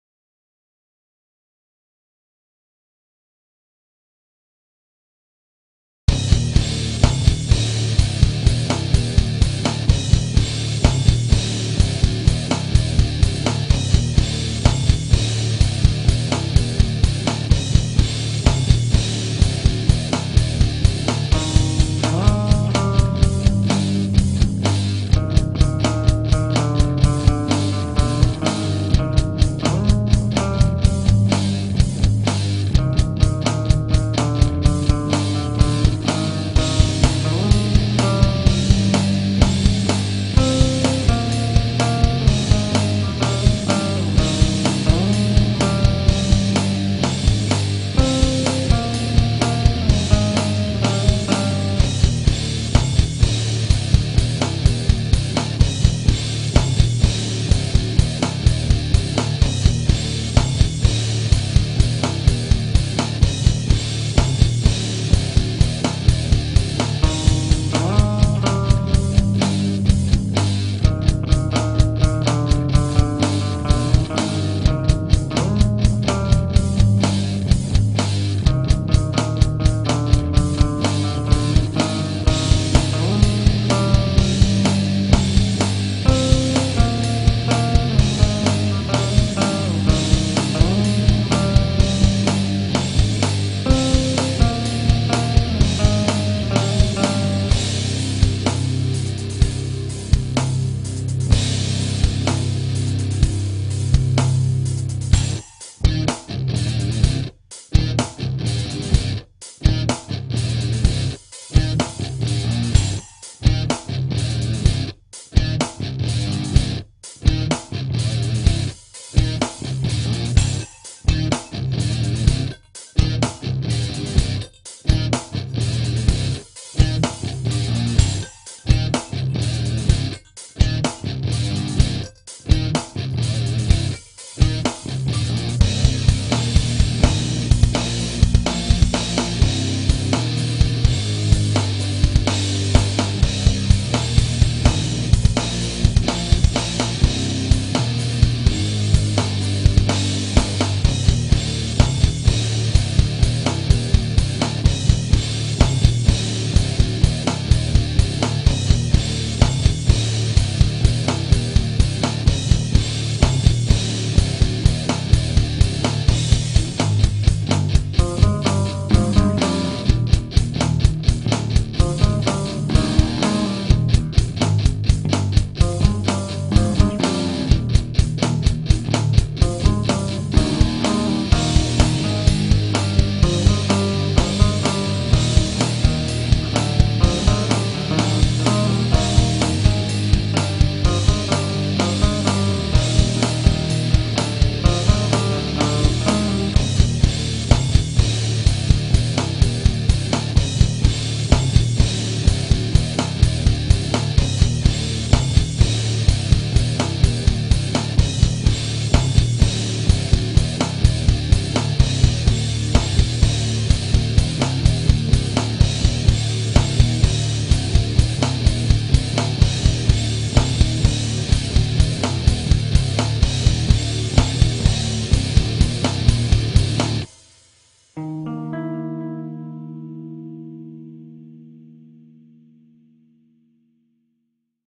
Drawing and Music with Cydedish 23
Drawing and Music by Cydedish

Original Instrumental Rock Metal

This project began as a weekly challenge to get the juices flowing.